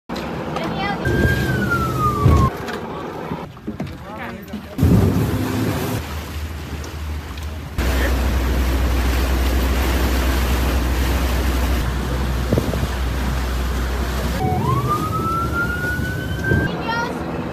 Migrants Interdicted at Sea!
Personnel from the U.S. Coast Guard Station Marathon, successfully detained 19 Cuban migrants about one mile off the coast of Marathon, Florida. And transferred them to the Coast Guard Cutter Jacob L. A. Poroo for repatriation to Cuba. The interdiction of migrants is among the 11 statutory missions delegated to the Coast Guard by Congress, empowering the military service to assume a prominent role in managing the continuous and noteworthy surge of migrants.